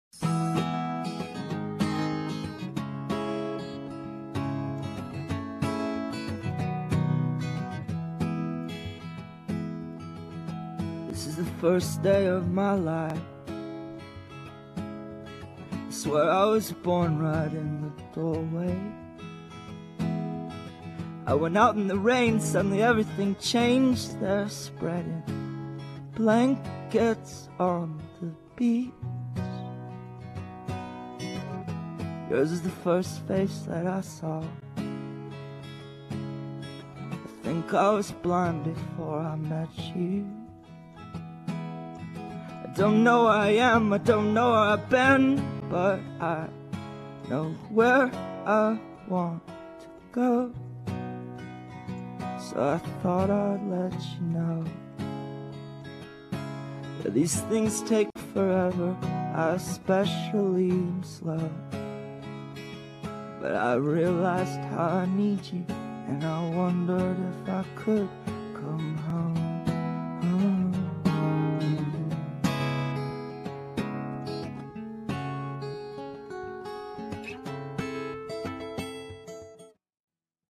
8mm Home Movie
Here's some old 8mm film of my sister and I.